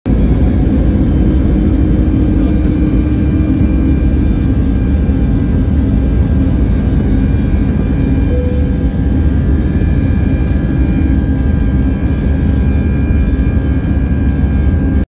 Teddy Bear Takes Flight: Adorable Adventure at Brussels Zaventem!
Enjoy the teddy bear’s exciting journey!

Experience the captivating takeoff of an airplane from Brussels Zaventem Airport, as the green screen comes to life with the presence of the beloved character Teddy bear 🧸 ! Join us in this exciting video and discover the magic behind creating this enchanting scene using green screen technology. Enjoy the thrilling blend of aviation and imagination in this amazing video.